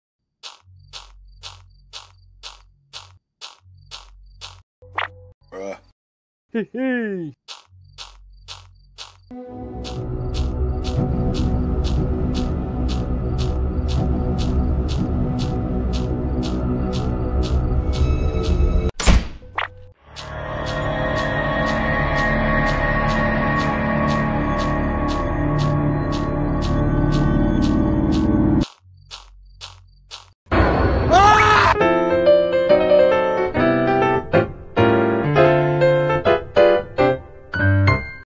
Mysterious Monster Cave
He spot a mysterious cave and goes inside where he finds strange things but what happens next is horrifying